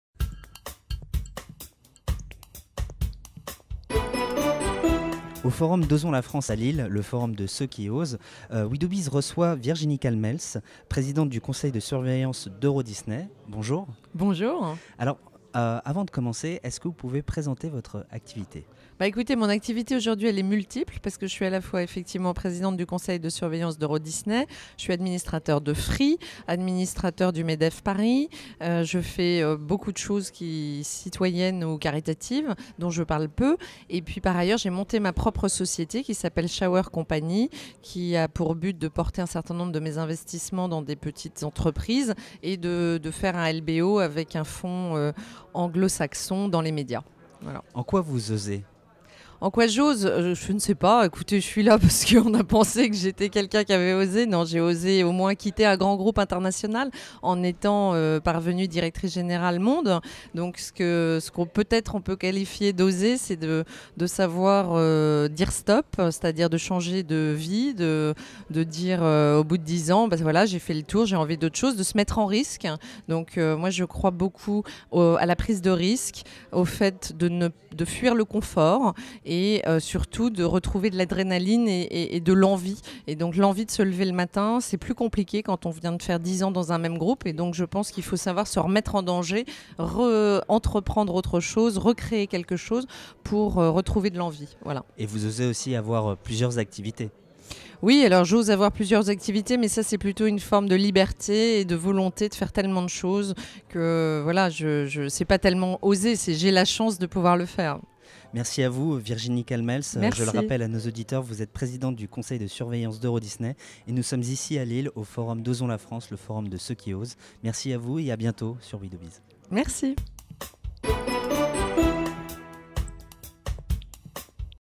Osons la France Lille 2013 : Virginie Calmels - Euro Disney
Virginie Calmels - Présidente du Conseil de Surveillance d'Euro Disney
Elle a osé quitter un groupe international leader pour entreprendre en France